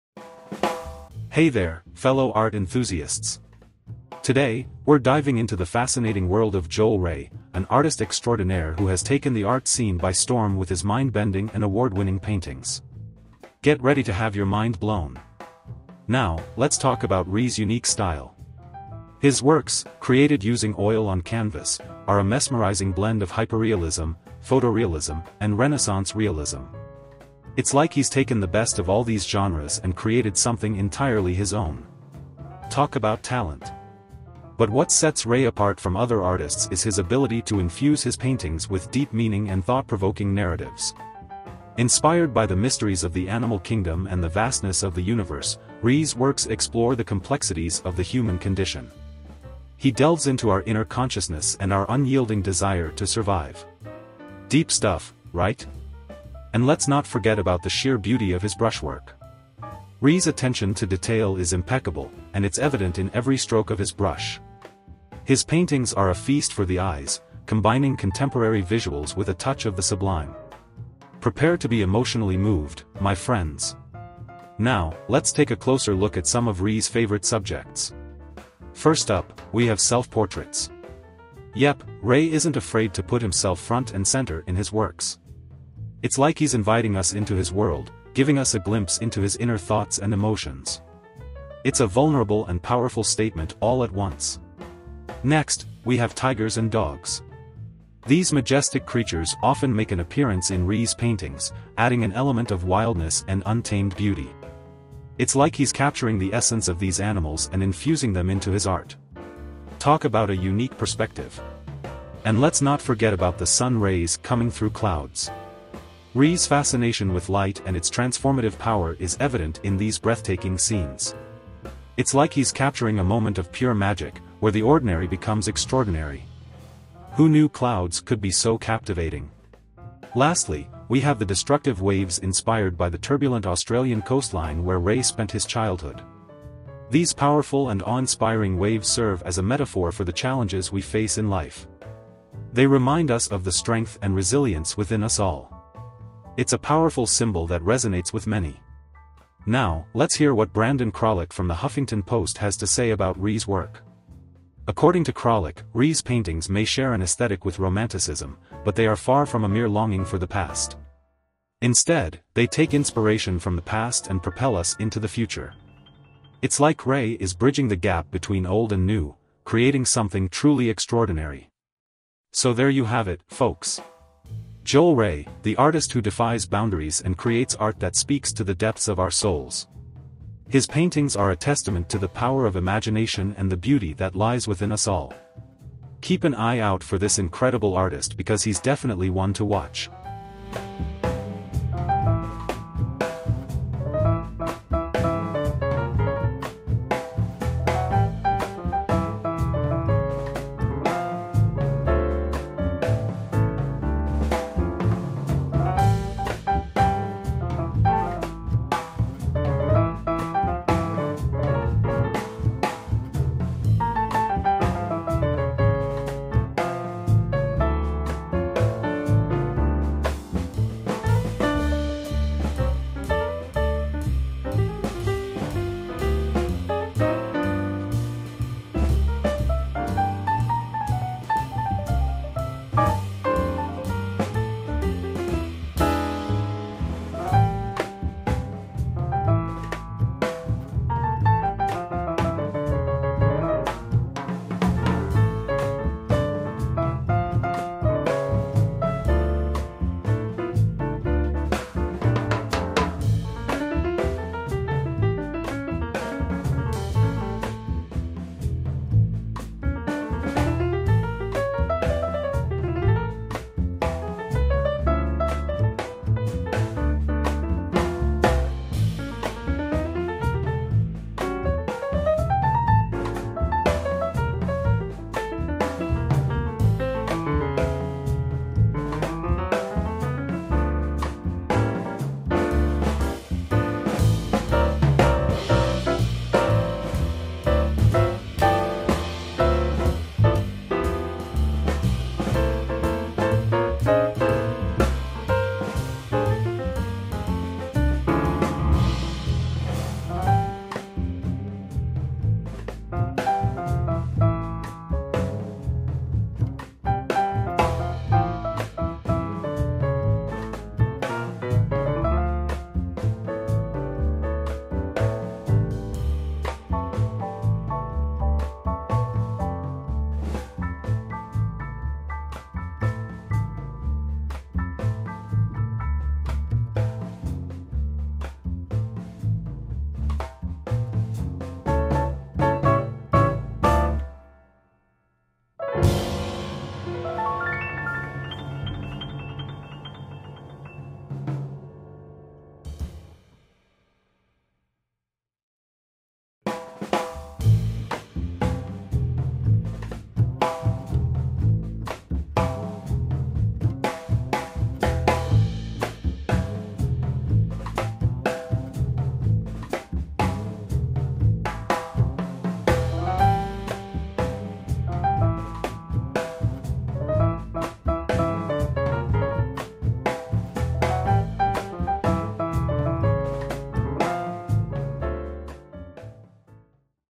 Joel Rea: Surreal Allegorical Paintings with a Message | Artist & Artworks
Welcome to Garden of Epicurus!
I will introduce the art and show the artworks of featured artists. Hope you like!

Discover the unique and recognisable style of multi award-winning artist Joel Rea, whose oil on canvas works combine genres of hyperrealism, photorealism, and Renaissance realism. Through his impeccable execution of brush work detail, Rea portrays social awareness and personal introspection, drawing inspiration from the mysteries of the animal kingdom and the Universe. His powerful imagery provokes deep emotional responses and presents messages embracing the ageless turmoil of human inner consciousness and our species’ unwavering desire to survive.